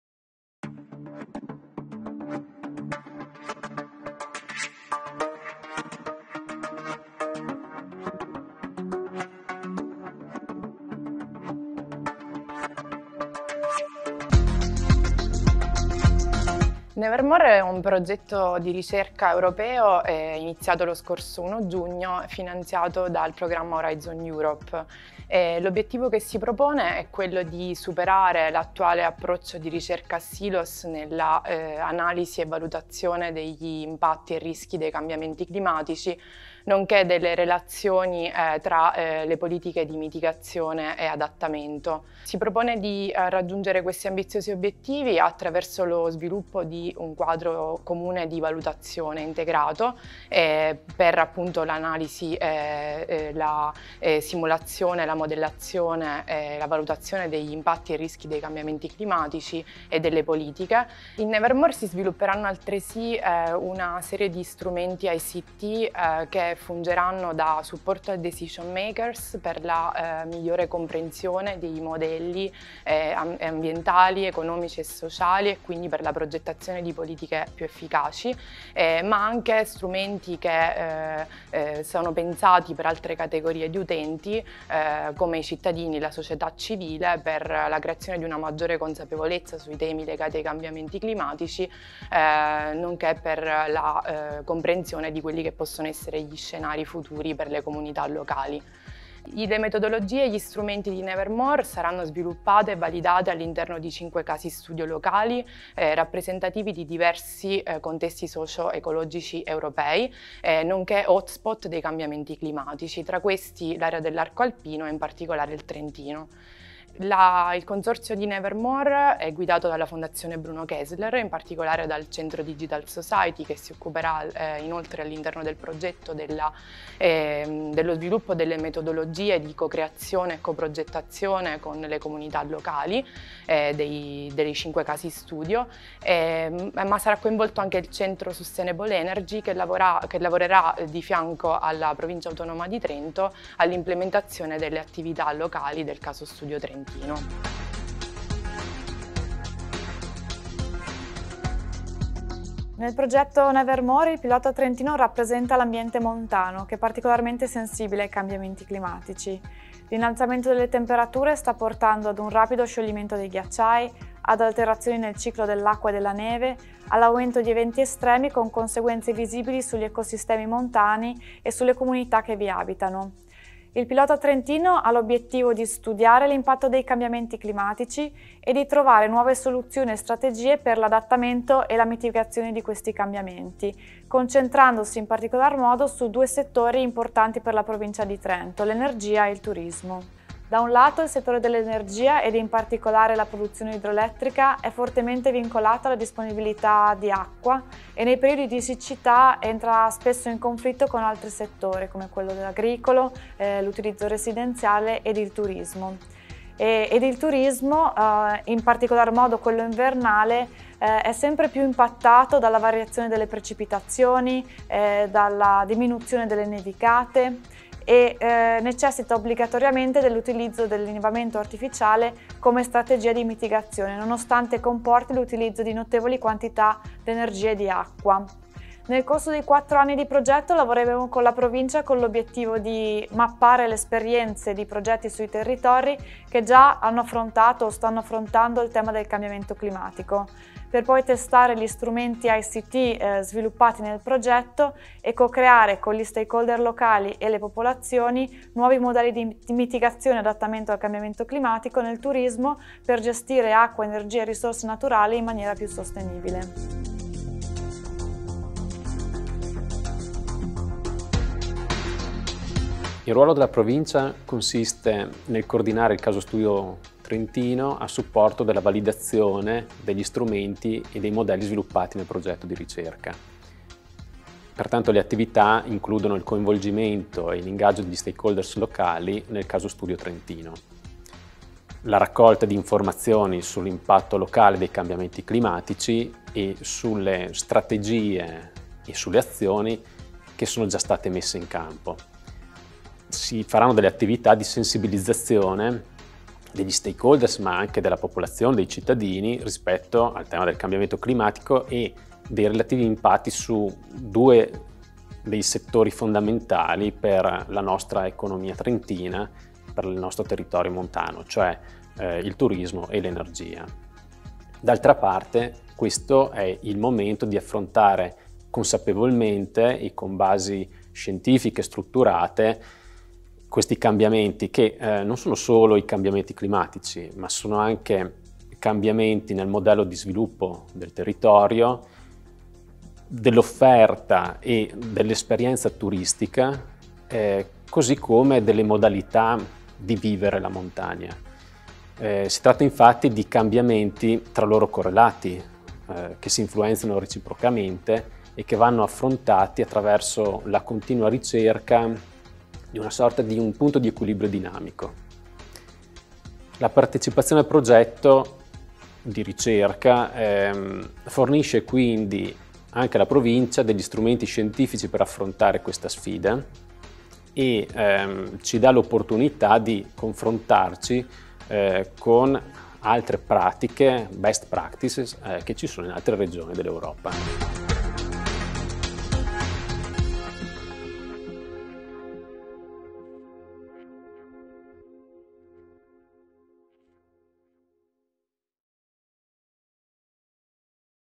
Progetto europeo NEVERMORE dedicato ai cambiamenti climatici
E' stato costituito il 02 marzo 2023, presso la Sede della Provincia, il Gruppo di Lavoro del progetto europeo NEVERMORE, un tavolo di stakeholder provinciali animato dal Servizio Turismo e sport della Provincia autonoma di Trento e supportato dalla Fondazione Bruno Kessler-FBK.